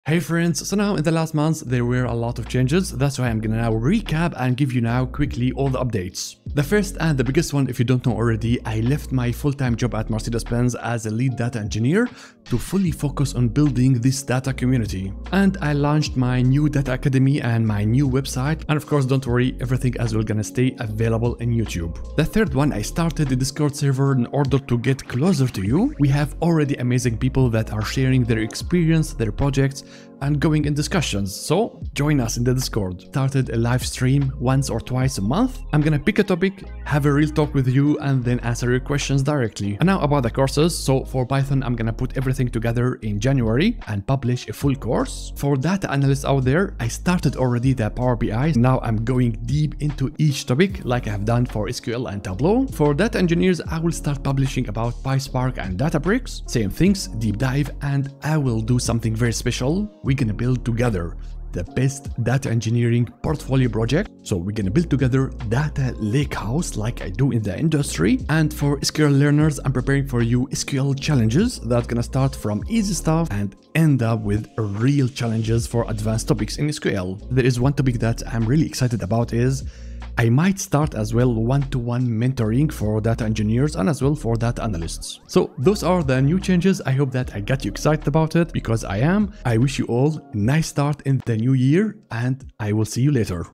10 Big Updates! The next Courses 🎉
Channel update where I share 10 big changes coming next to Data With Baraa.

*MY COURSES*
To get certified, better clean structure & progress, support me: